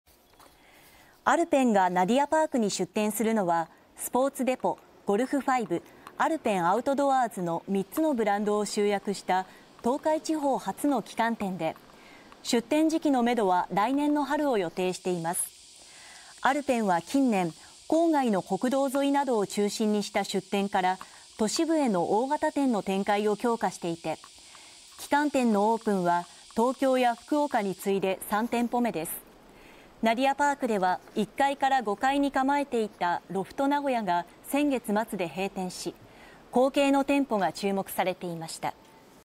ロフト名古屋が6月末閉店…栄のナディアパークに『アルペン』が旗艦店を出店へ 東京や福岡に次ぐ3店舗目
名古屋市中区に本社を置くスポーツ用品販売大手「アルペン」が、名古屋・栄 の#ナディアパーク に旗艦店を出店することが分かりました。

アルペンがナディアパークに出店するのは、「スポーツデポ」「ゴルフ5」「アルペンアウトドアーズ」の3つのブランドを集約した東海地方初の旗艦店で、出店時期の目途は2024年の春を予定しています。

アルペンは近年、郊外の国道沿いなどを中心にした出店から、都市部への大型店の展開を強化していて、旗艦店のオープンは東京や福岡に次いで3店舗目です。

ナディアパークでは、1階から5階に構えていた「ロフト名古屋」が2023年6月末で閉店し、後継の店舗が注目されていました。

2023年7月25日放送